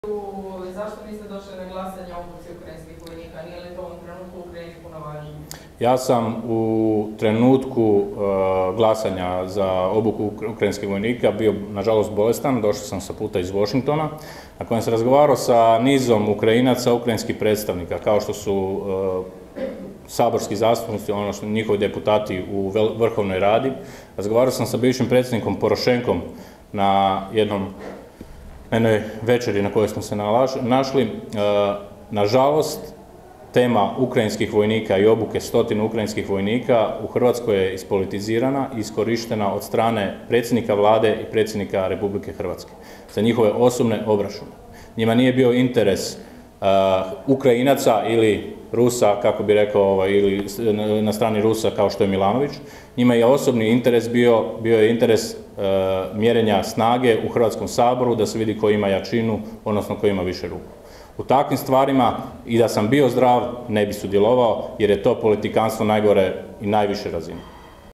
Milanović Litre o tome zašto nije glasovao za obuku ukrajinca
Na konferenciji za medije održanoj u Saboru o aktualnim temama govorio je Marko Milanović Litre.